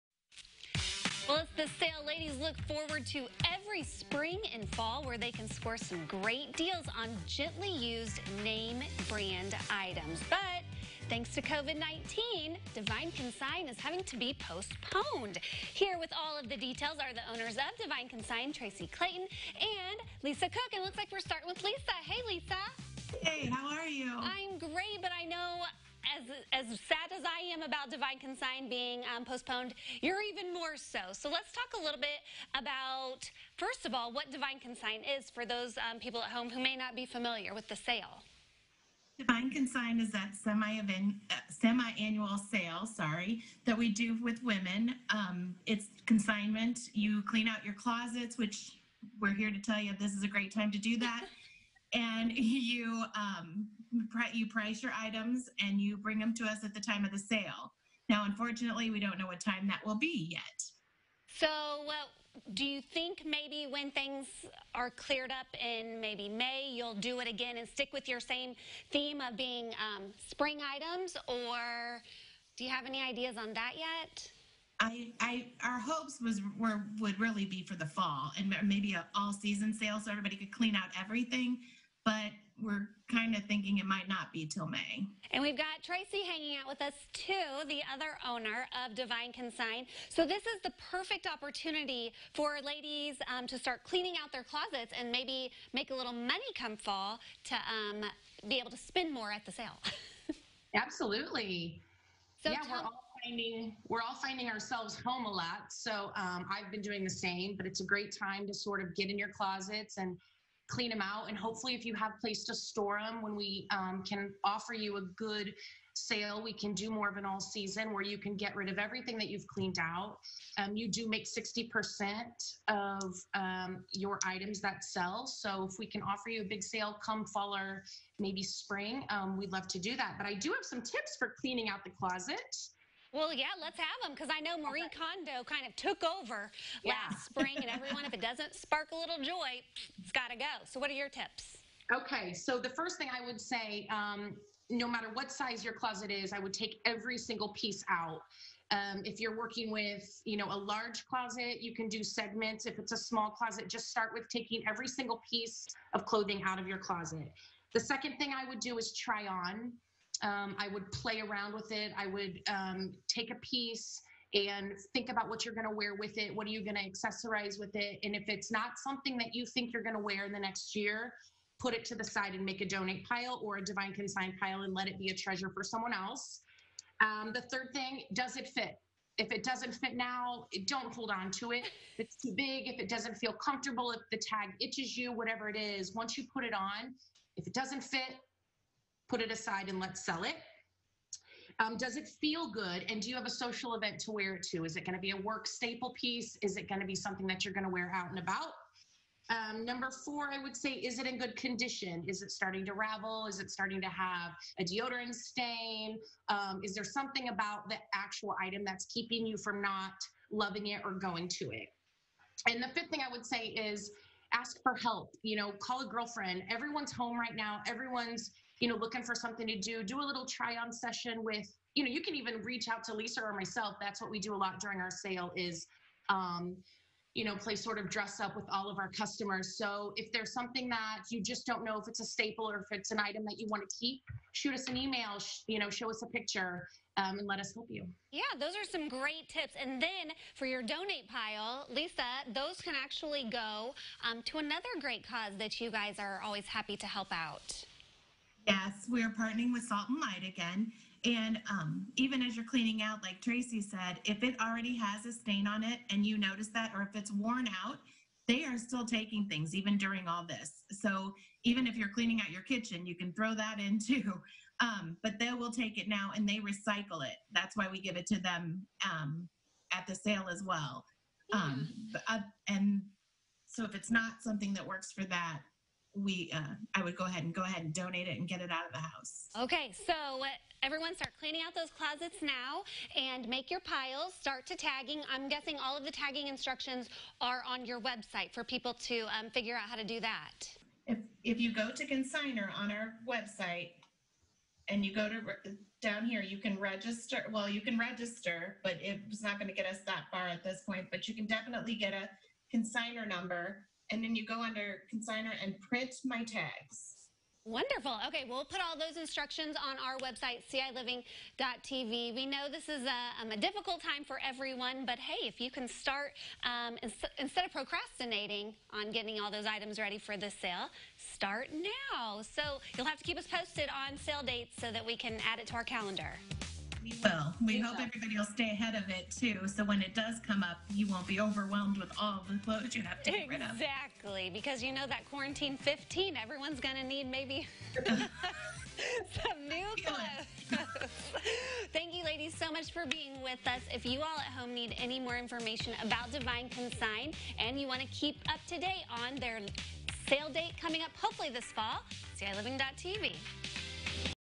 Tips from Divine Consign on cleaning out your closets
We're getting tips on how to clean out your closet from Divine Consign.